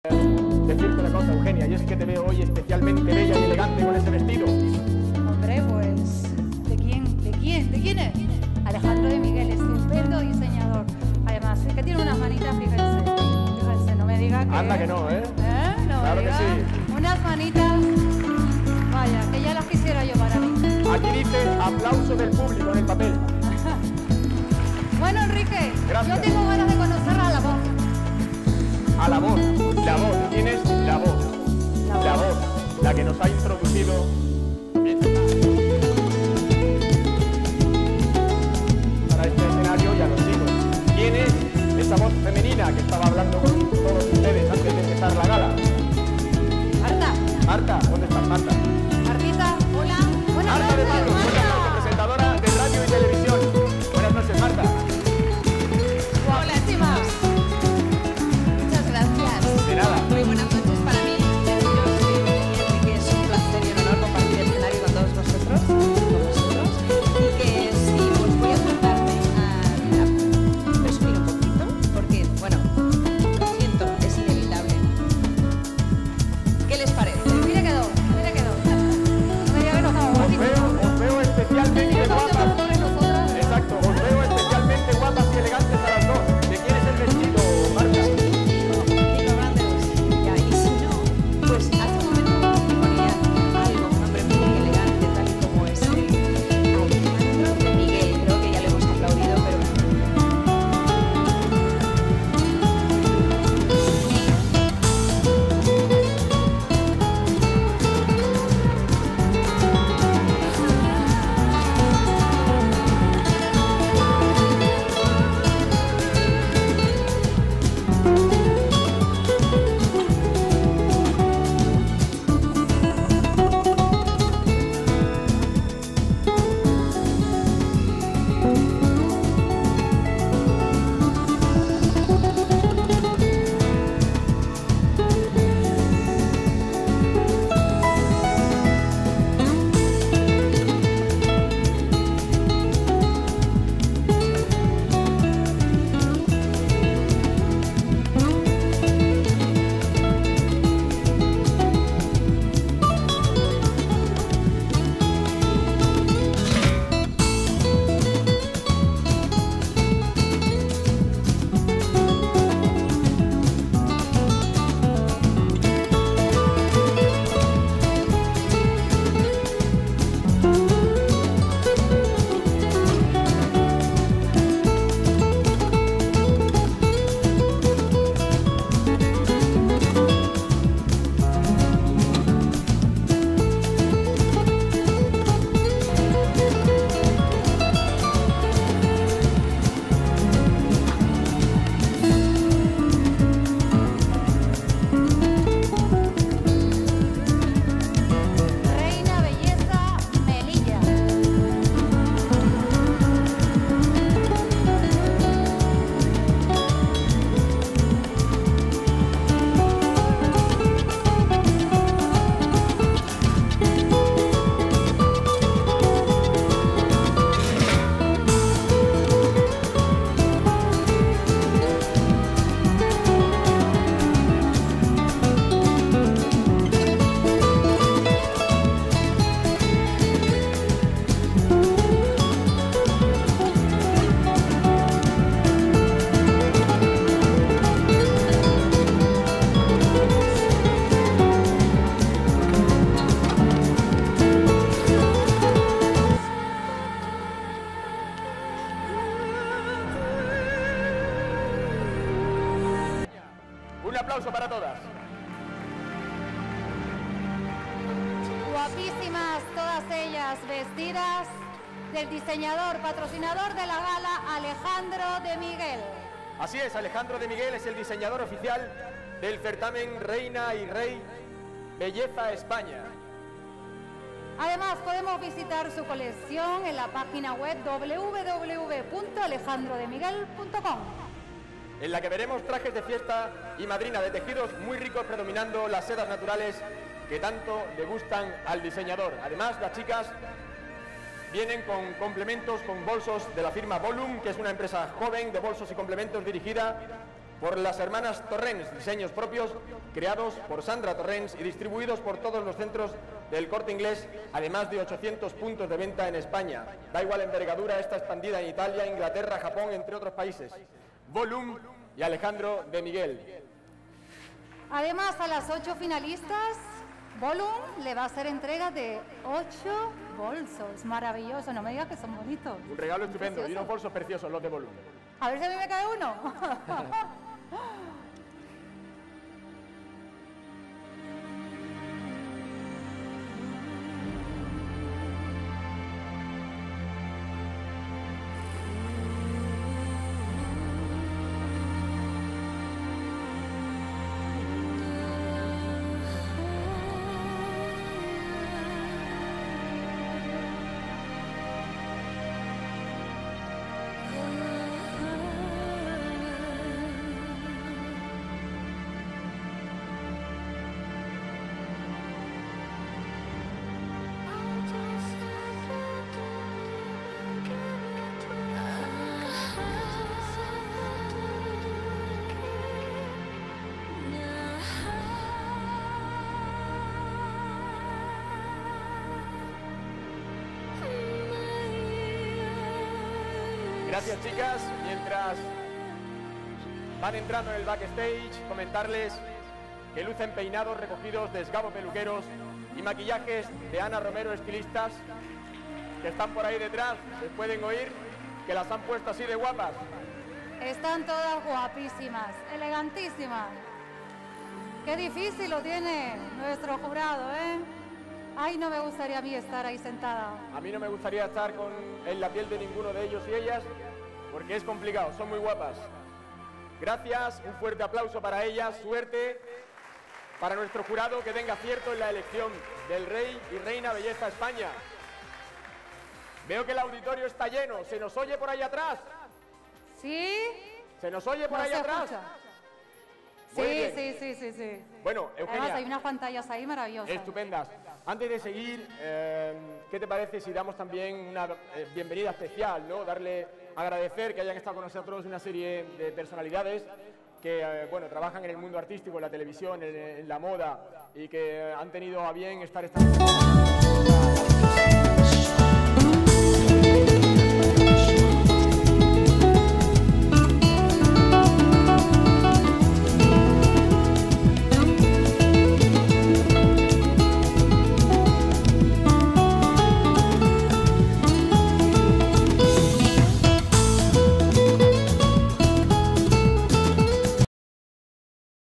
Desfile de Gala Reinas Belleza 2008
En este video podemos ver desfilar a las 17 Reinas de Belleza autonómicas más las representantes de Ceuta y Melilla, la gala fue presentada por Eugenia Santana y Enrique Miranda y contaron la ayuda especial de la popular actriz y presentadora Marta de Pablo. Desfilan vestidos del gran diseñador manchego Alejandro de Miguel , al acto acudieron numerosos rostros conocidos además de varias  miss España y mister, así como alguna Linda de España.Gala celebrada en el teatro del hotel Diamante beach de Calpe.